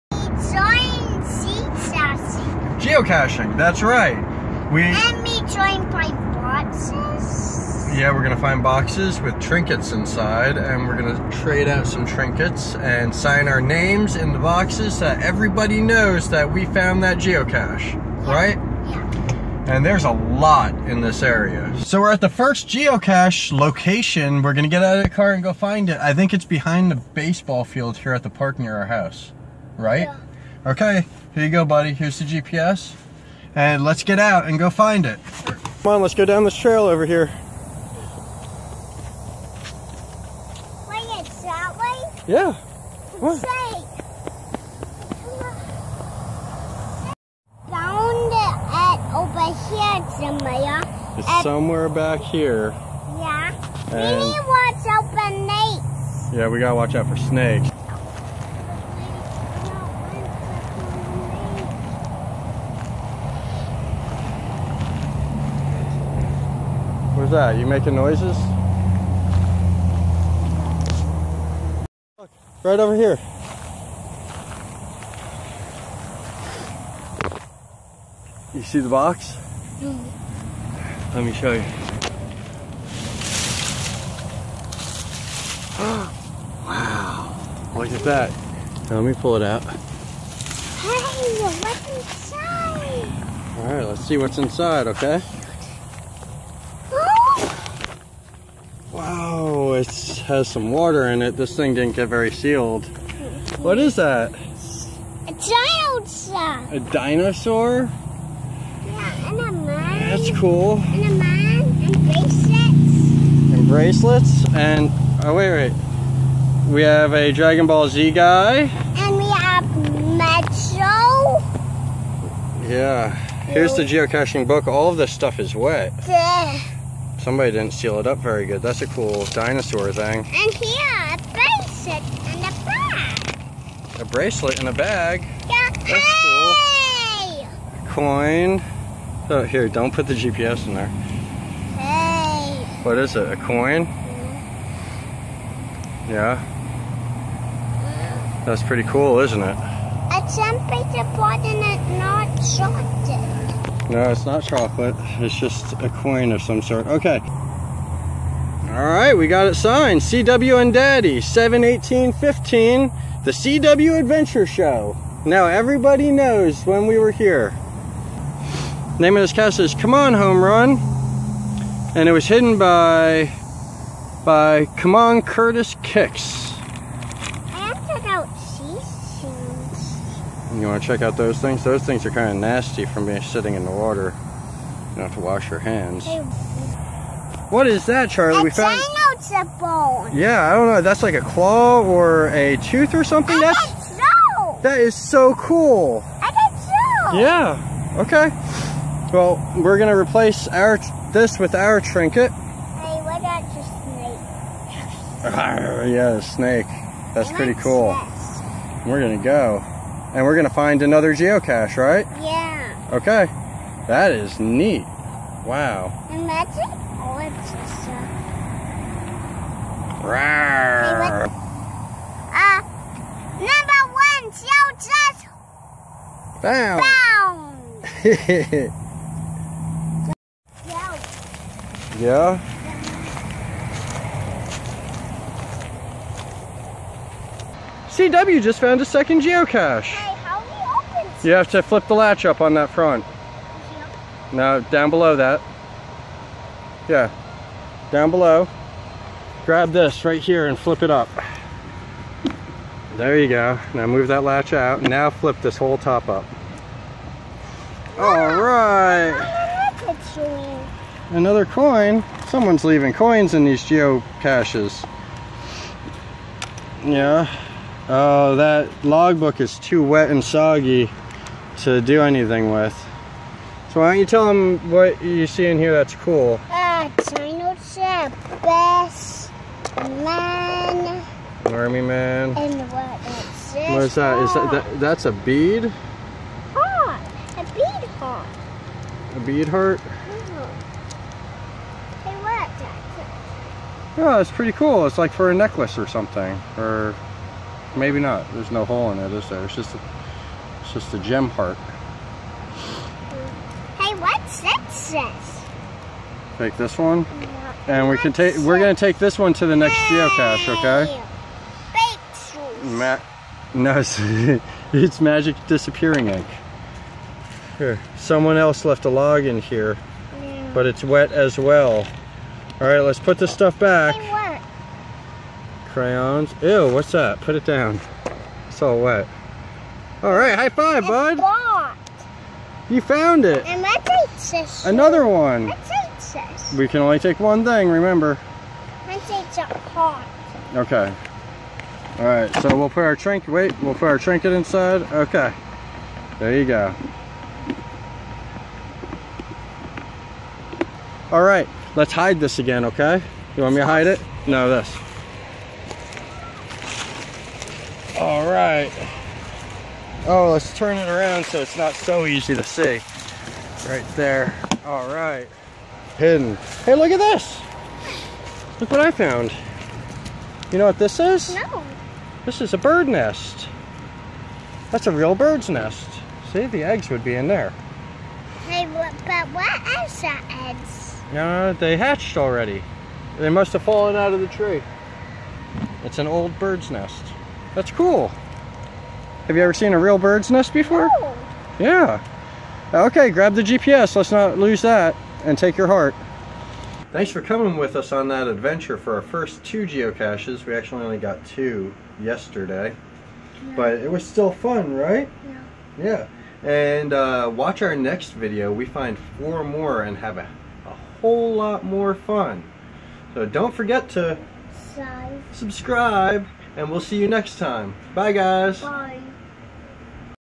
Our First Geocache Adventure
CW and Daddy go geocaching for the first time! They find only two Geocaches but decide it's a lot of fun!